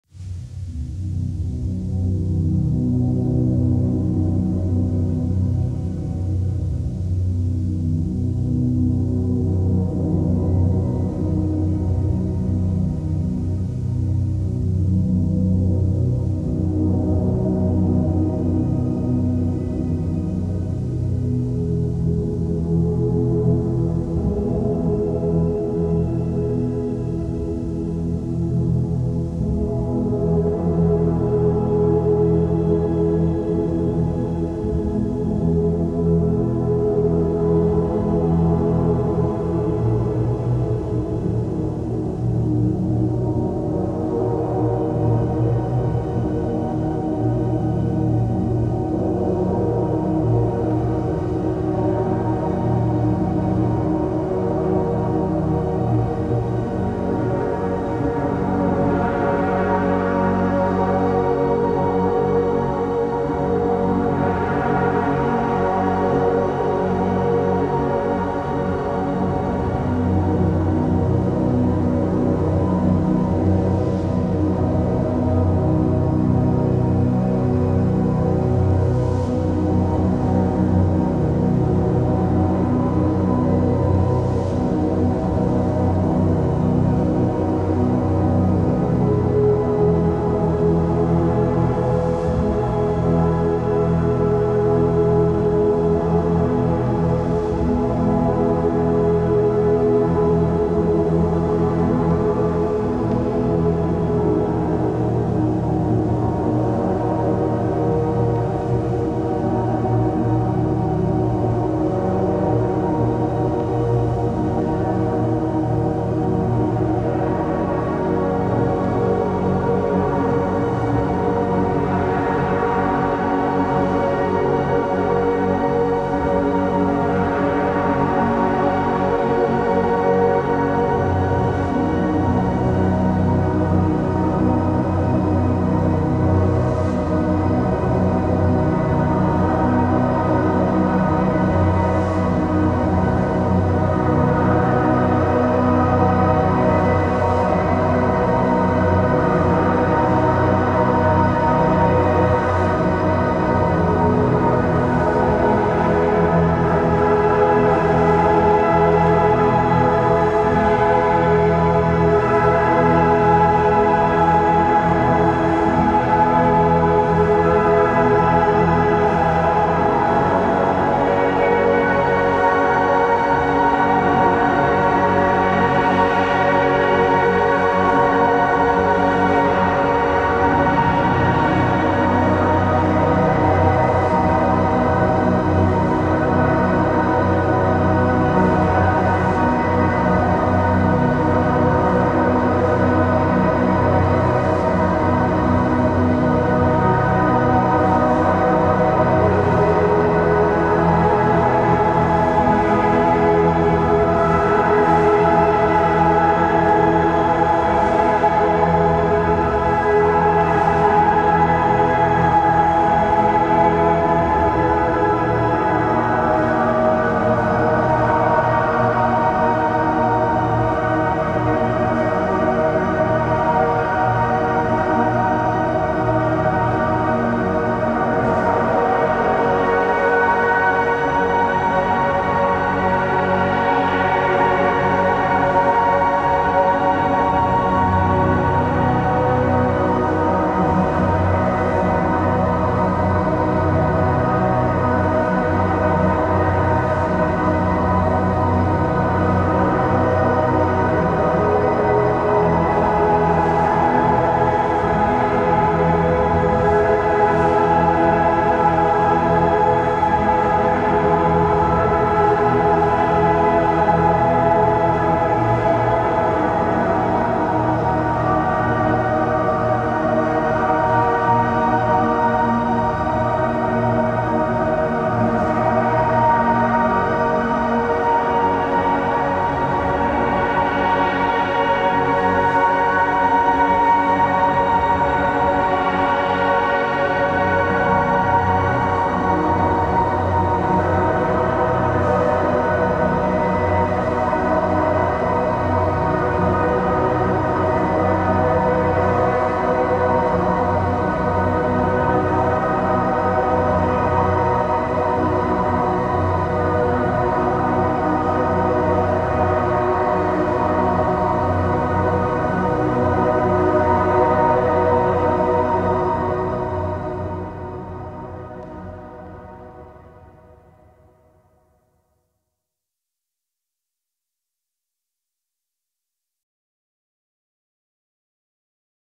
Live Ambient Tape Loop on Cassette Recorder + Microcosm, Waldorf 2 Pole, Death by Audio Rooms
Gear used:

Yamaha Multitrack MT120
Microcosm Hologram
Neunaber Immerse Shimmer Reverb
Death by Audio Rooms
Waldorf 2-Pole Filter
Oto Boum

4-Tracks Tape Loop on Yamaha Cassette Recorder + Microcosm Hologram, Oto Boum, Death by Audio Rooms